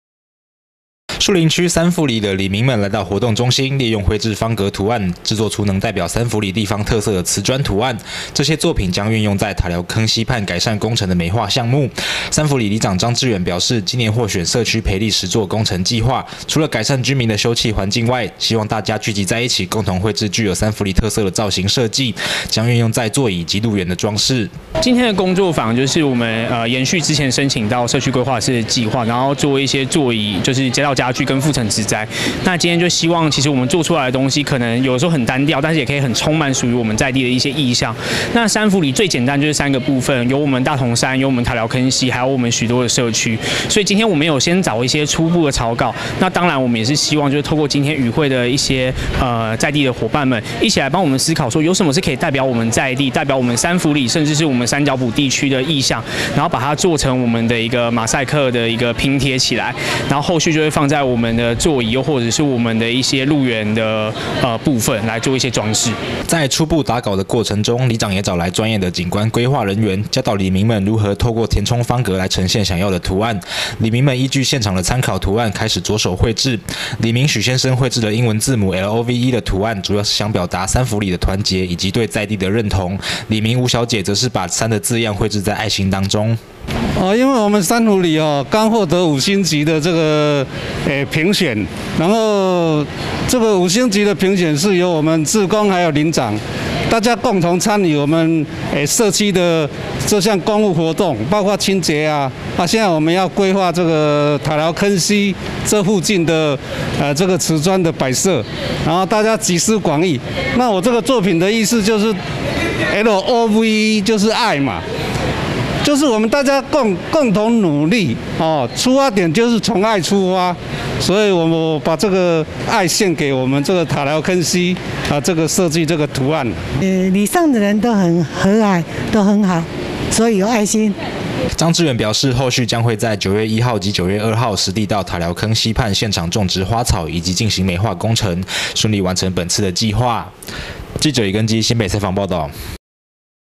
環境 社區 |塔寮坑溪畔改善計劃 里民共繪造型裝飾
塔寮坑溪畔改善計劃 里民共繪造型裝飾
樹林區三福里獲選112年度社區培力與實作工程計劃，針對塔寮坑溪畔週遭的公共空間進行改善與美化，整個改善工程邀請里民們一同來為美化設計提供意見，以馬賽克磁磚拼貼出具有三福里特色的圖案，實踐公民參與的精神。

樹林區三福里的里民們來到活動中心，利用繪製方格圖案創作出能代表三福里地方特色的磁磚圖案，這些作品將運用於塔寮坑溪畔改善工程的美化項項目，三福里里長張致遠表示，今年獲選社區培力與實作工程計劃，除了改善居民的休憩環境外，希望將大家聚集起來共同繪製具有三福里特色的造型設計，將用在座椅以及路緣的裝飾。

在初步打稿的過程中里長也找來專業的景觀規劃人員，教導里民們如何透過填充方格來呈現想要的圖案，里民們依據現場的參考圖案，開始著手繪製，里民許先生繪製了英文字母love的圖案主要是想表達三福里的團結與對在地的認同，里民吳小姐則是把三的字樣繪製在愛心當中。

張致遠表示後續將會在9月1日及2日實地到塔寮坑溪畔現場種植花草以及進行美化工程，順利完成本次的計畫。
或「北健活力頻道」